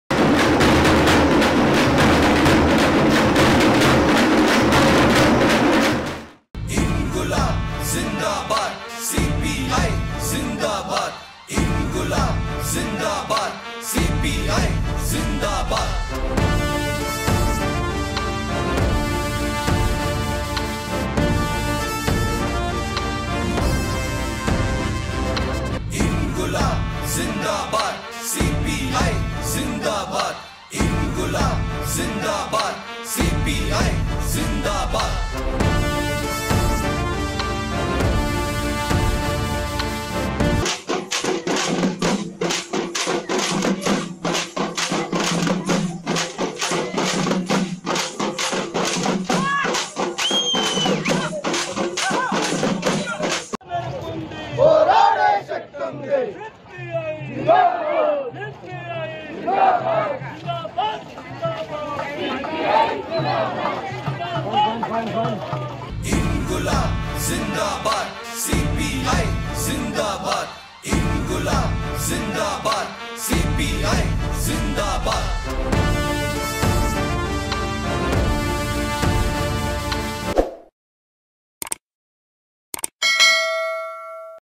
இந்திய கம்யூனிஸ்ட் கட்சியின் 24 வது மாநாடு - விஜயவாடா நகரை சிவைக்க வைத்த மாபெரும் பேரணி
இந்தியக் கம்யூனிஸ்ட் கட்சியின் அகில இந்திய மாநாடு விஜயவாடாவில் அக்டோபர் 14 முதல் 18 ஆம் தேதி வரை நடைபெற்றது. பல கிலோ மீட்டர் தொலைவுக்கு அணிவகுத்துச் சென்ற இந்த மாநாடு விஜயவாடா நகரையே சிவக்க வைத்த‍து.

மேலும் வீடியோக்கள் பார்க்க ஜனசக்தி தொலைக்காட்சி..